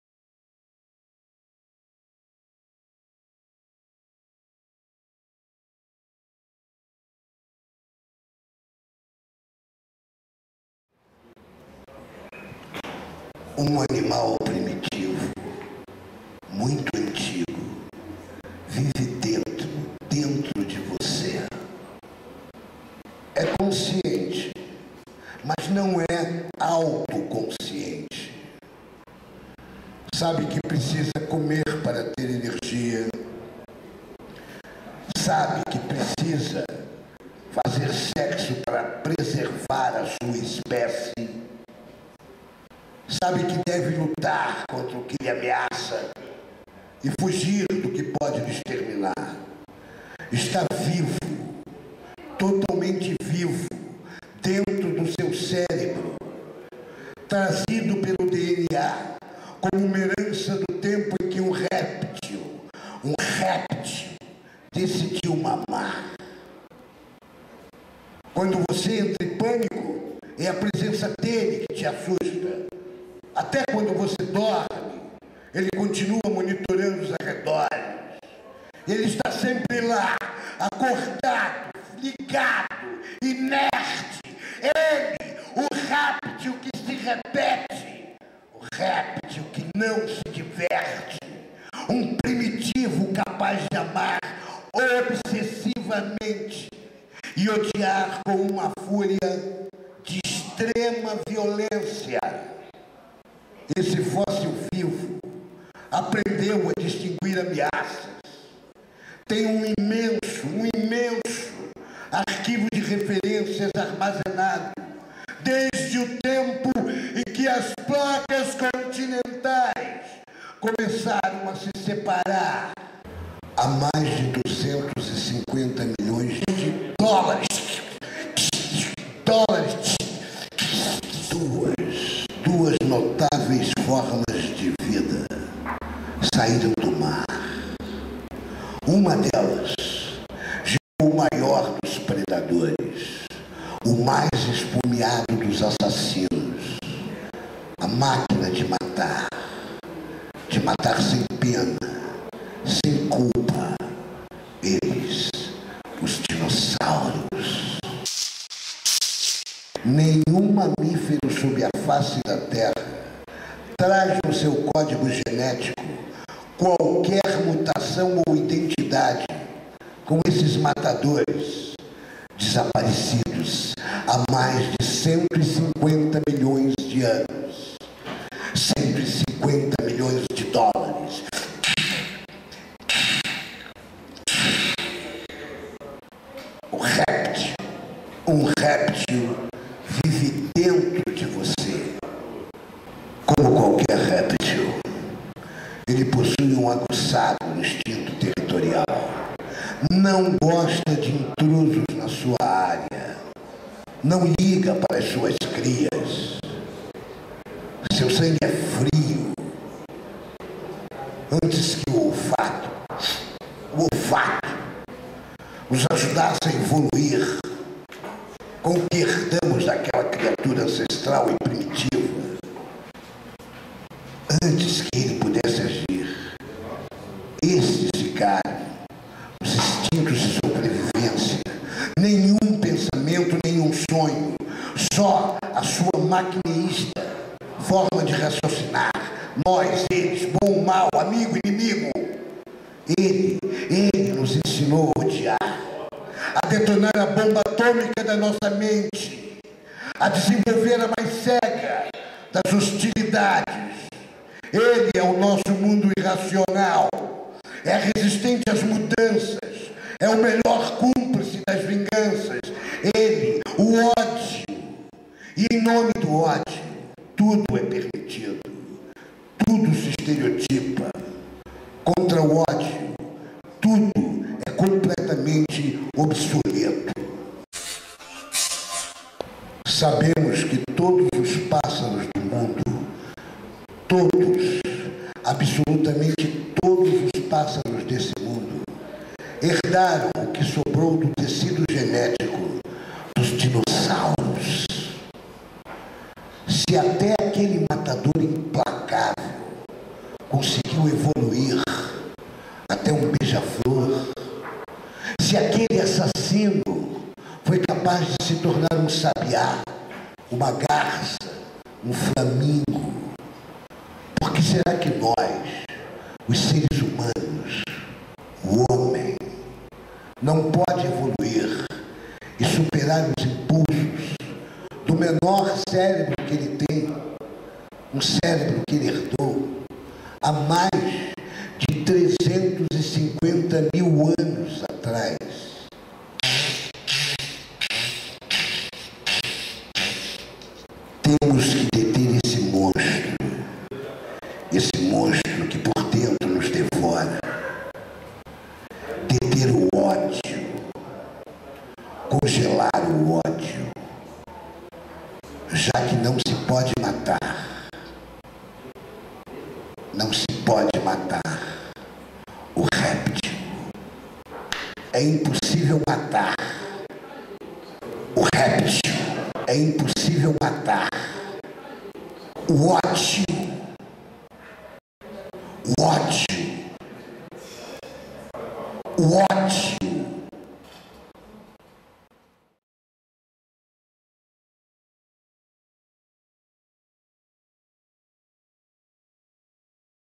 TAVINHO PAES: O Réptil [action-poem]
O poema O RÉPTIL, de Tavinho Paes, publicado no booklet PIC NIC MIX (lançado no Festival), foi apresentado dia 01 de dezembro, às 22:30h, com a performance HAIR BACK, item do trabalho "FACE in TIME" em desenvolvimento desde 2000.
Constitui um "action-poem" (poema em movimento) uma vez que a imagem do poeta é modificada em tempo real com a participação especial do grupo BACANETZ*: Ana de Lucca, Taciana (hair-cut machine) e Patrícia Magal.

* vide video-entrevista no box My Recente Rattings